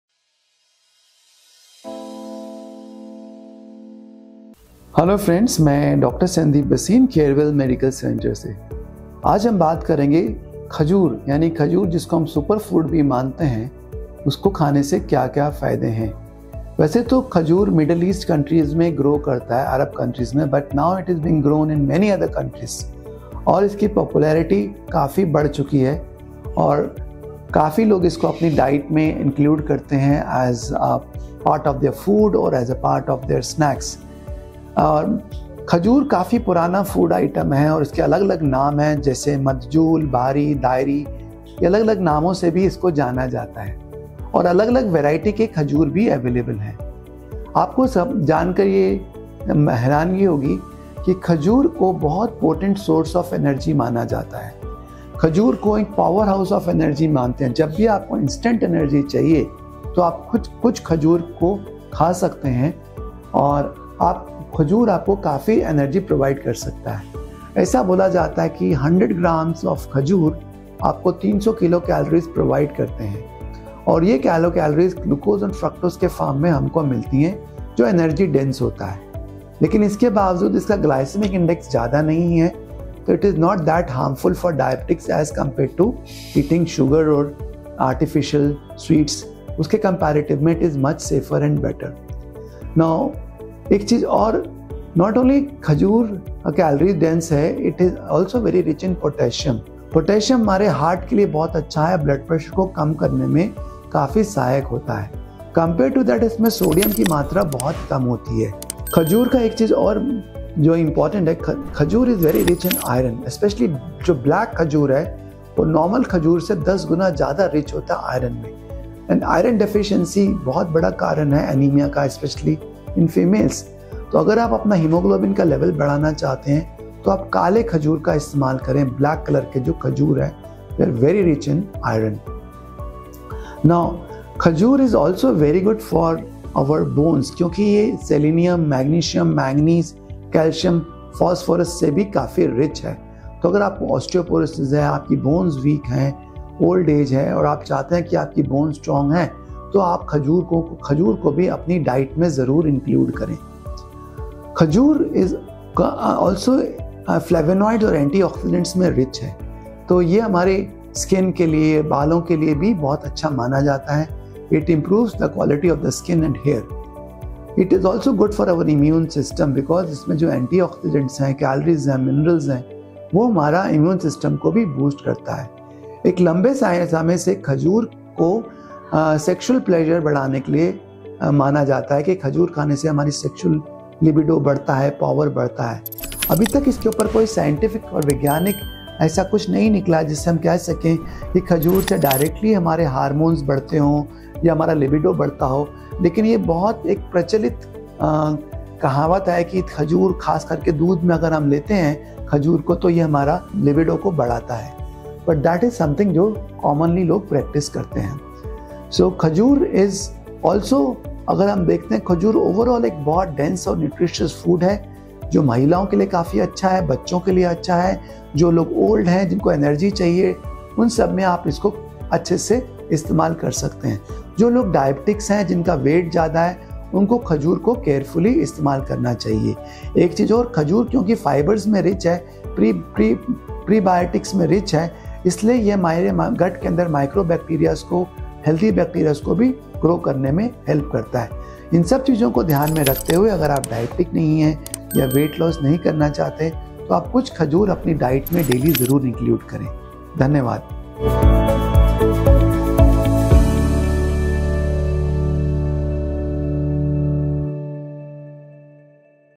खजूर खाने के फायदे (Health Benefits of Eating Dates) | Care Well Medical Centre
खजूर खाने के अविश्वसनीय फ़ायदों से आप एक स्वस्थ व्यक्ति बन सकते हैं! 🌟 इस वीडियो में, केयर वेल मेडिकल सेंटर उन रहस्यों पर गहराई से प्रकाश डालता है कि कैसे खजूर आपकी सेहत को बदल सकता है। ऊर्जा बढ़ाने से लेकर पाचन में सुधार तक, हम इस सुपरफूड के चमत्कारों को उजागर करते हैं। 🌰✨बेहतर स्वास्थ्य की यात्रा के लिए अभी सदस्यता लें!
Unlock a healthier you with the incredible benefits of eating dates! 🌟 In this video, Care Well Medical Centre delves deep into the secrets of how dates can transform your well-being. From boosting energy to improving digestion, we unravel the wonders of this superfood.

Care Well Medical Centre
House No. 1, Nri Complex
Chitranjan Park

Thanks & Regards,
Dr. Sandeep Bhasin